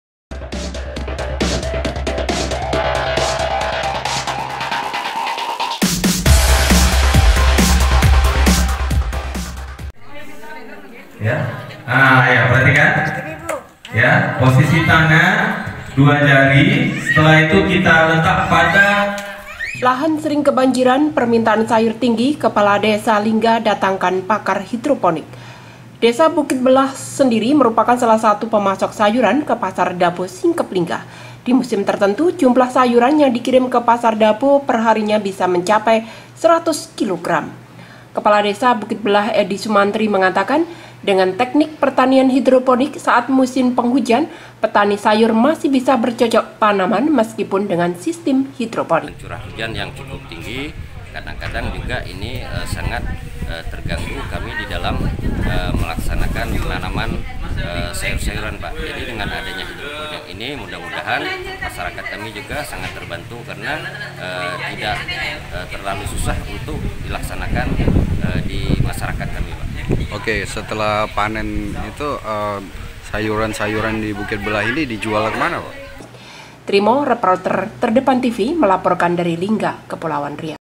Kades Bukit Belah Datangkan Pakar Hidroponik
TERDEPAN.CO.ID, LINGGA- Lahan sering kebanjiran, permintaan sayuran tinggi kepala desa lingga datangkan pakar hidroponik

Desa Bukit Belah sendiri merupakan salah satu pemasok sayuran ke pasar Dabo Singkep Lingga. di musim tertentu, jumlah sayuran yang dikirim ke pasar Dabo, perharinya bisa mencapai 100 kilo.

Kepala Desa Bukit Belah, Eddi Sumantri, mengatakan, dengan teknik pertanian hidroponik saat musim penghujan’ petani sayur masih bisa bercocok tanaman meskipun dengan sistem hidroponik.

Trimo Reporter Terdepan TV Melaporkan Dari Lingga  Kepulauan Riau